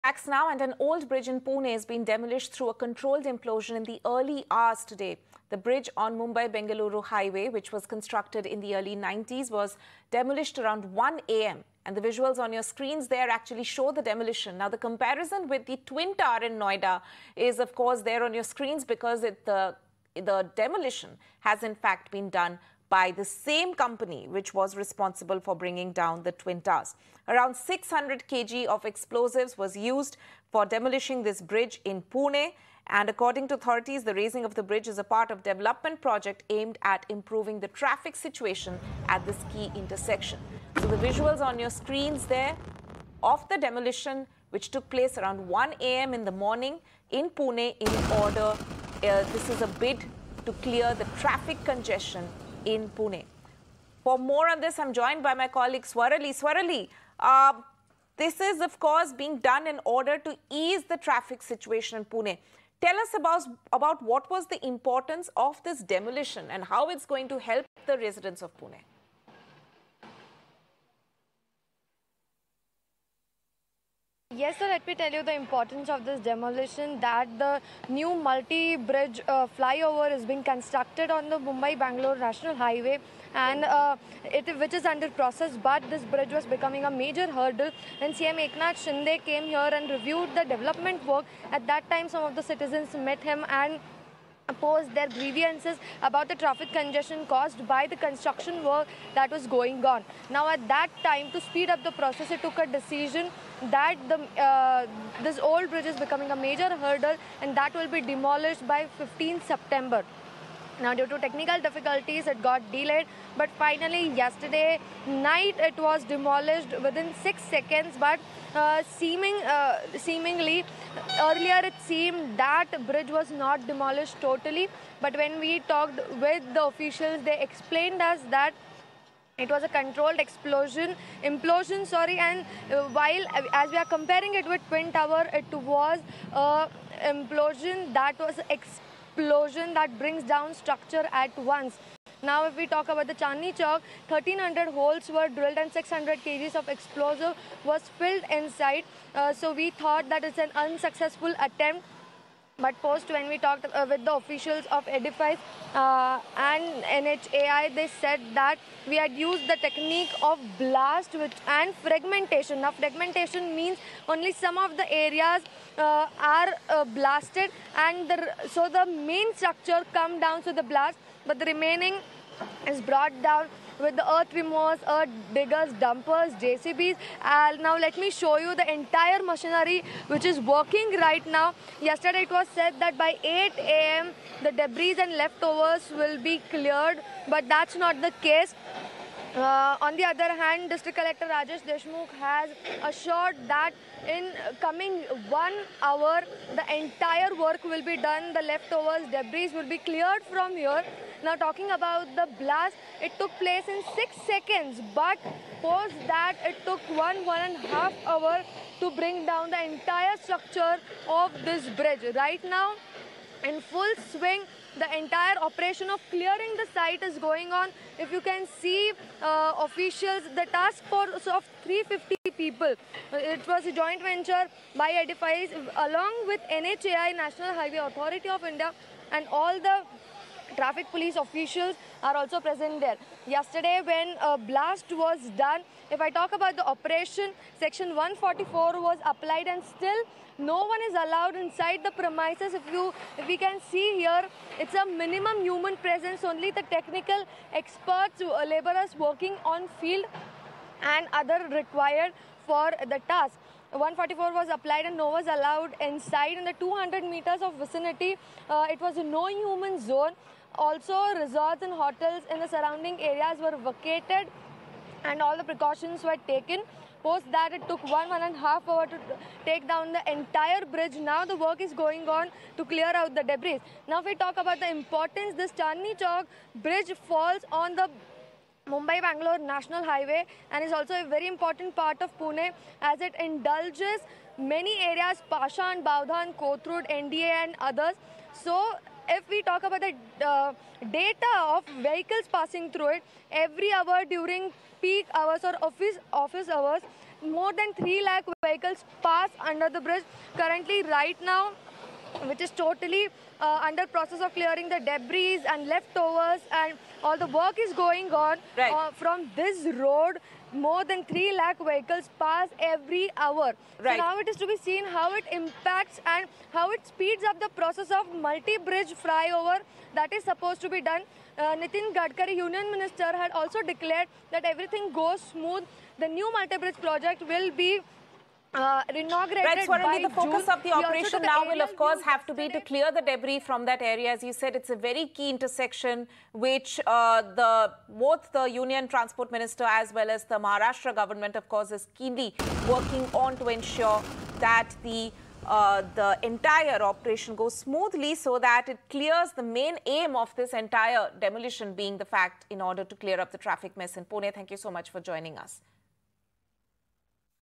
Pune News: Chandni Chowk Bridge Demolished In Midnight, 600 Kgs Of Explosives Used | English News
Pune News: An old bridge was brought down via a mega demolition operation to clear traffic congestion in the city at midnight. 600 kg explosives were used to demolish the old bridge. A controlled explosion early on Sunday morning destroyed a historic bridge in Pune's Chandani Chowk neighborhood. The bridge across the NH4 in the Chandani Chowk neighborhood of the city was constructed in the early 1990s. Several earthmover equipment and vehicles were put to use after the bridge was destroyed at 1 am on Sunday to bring down the remaining steel structures and lift the wreckage. The destruction was a part of a large-scale Chandni Chowk construction project intended to improve the flow of traffic at the important intersection. Edifice Engineering was contracted to do the work. Tune in for more such updates and news.